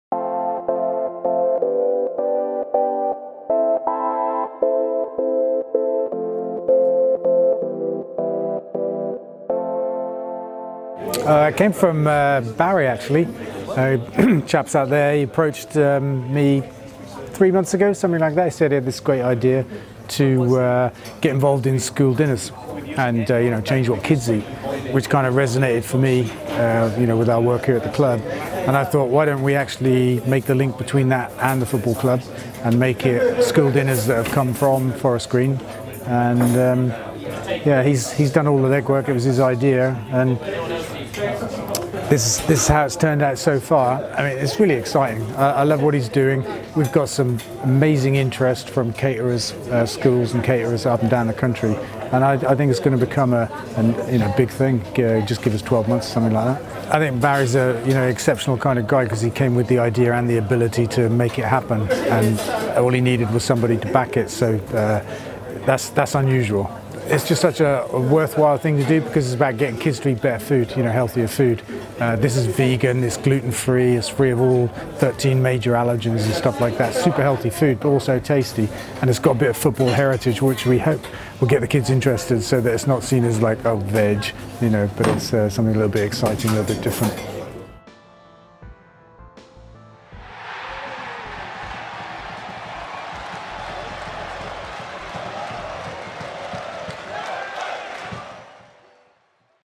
FGR launch Little Green Devils
Yesterday we launched Little Green Devils - our new range of vegan school meal products.

Hear from Dale about how the project came about and our exciting plans for the future.